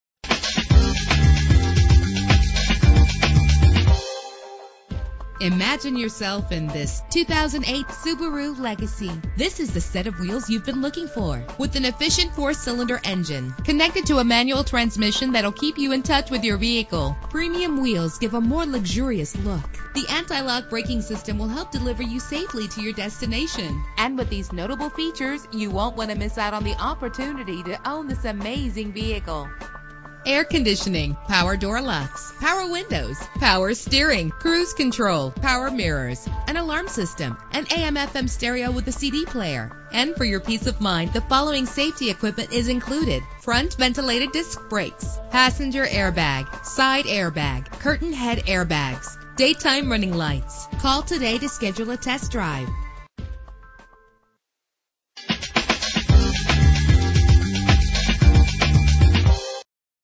2008 Subaru Legacy - Terre Haute IN
Year: 2008
Make: Subaru
Model: Legacy
Trim: 2.5i
Engine: 2.5 liter H4 16 valve
Transmission: Manual
Color: Black
Mileage: 60443
Address:
105 West Mayfair Drive